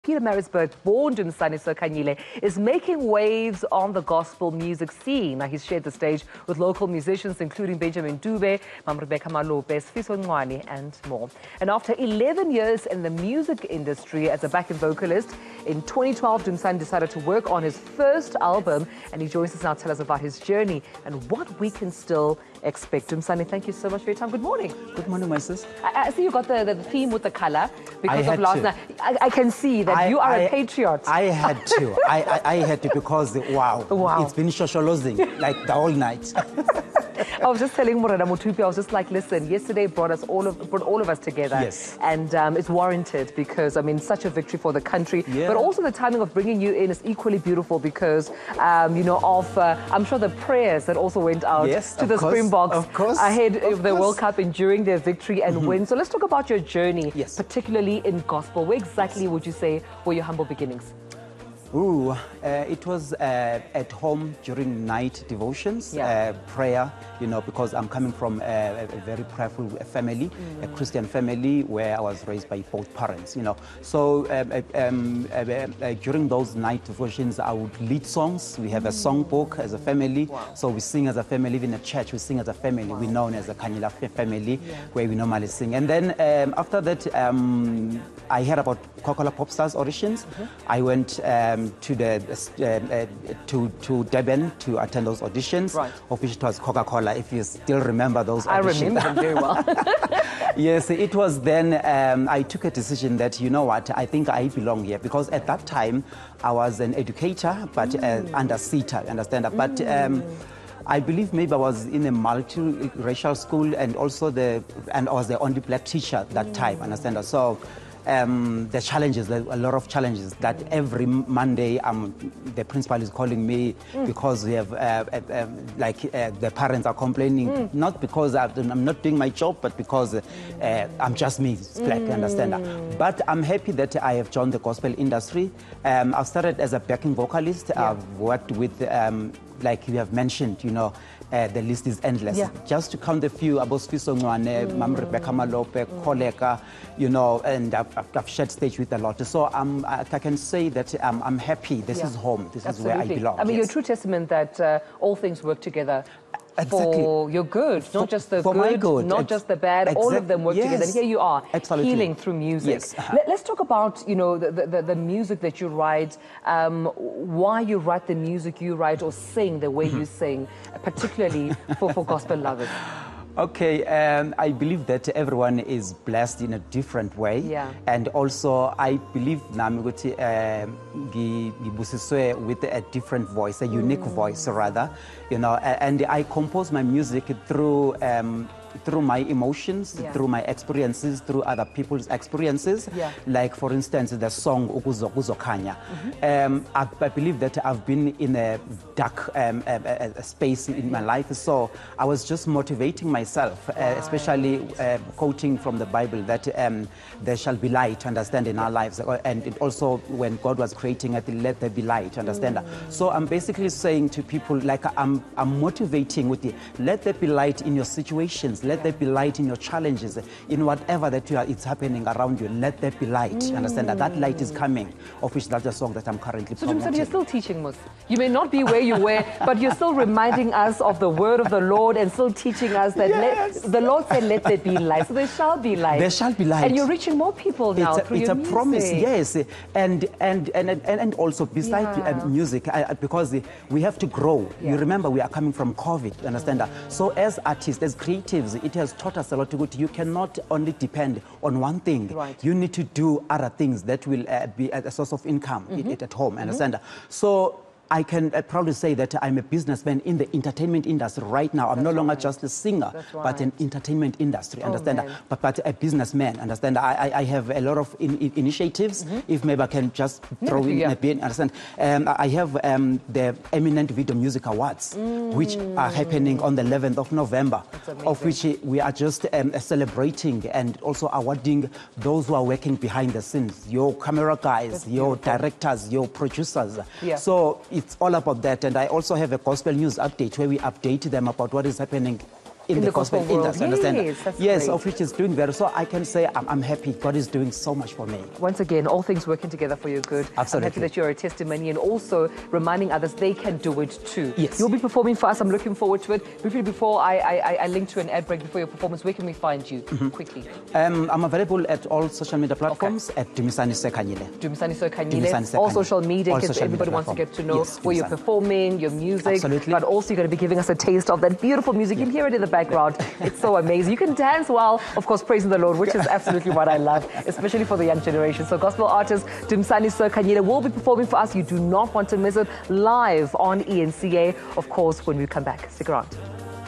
PMB born Dumisani Sir Khanyile making waves on the gospel music scene
Pietermaritzburg born Dumisani Sir Khanyile is making waves on the gospel music scene. He's shared the stage with local musicians including Benjamin Dube, Rebecca Malope, Sfiso Ncwane and more. After 11 years in the Music Industry as a Backing Vocalist, in 2012 Dumisani decided to work on his first album.